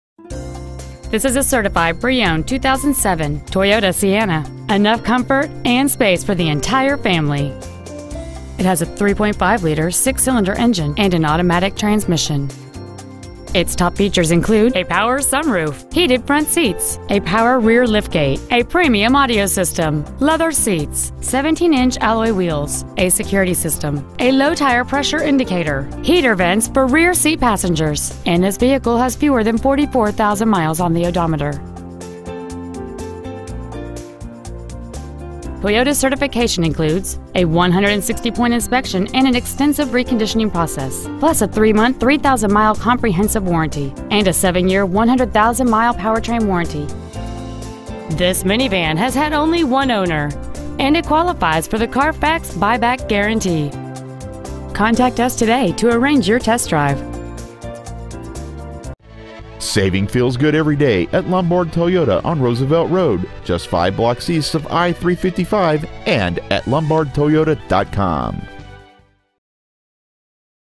2007 Toyota Sienna Certified IL Lombard
Year : 2007

 Make : Toyota

 Model : Sienna

 Engine : Gas V6 3.5L/

 Trans . : Automatic

 Exterior : Arctic Frost Pearl

 Miles : 43,679

 Interior : Stone

 Lombard Toyota

 725 W. Roosevelt Road

  , IL 60148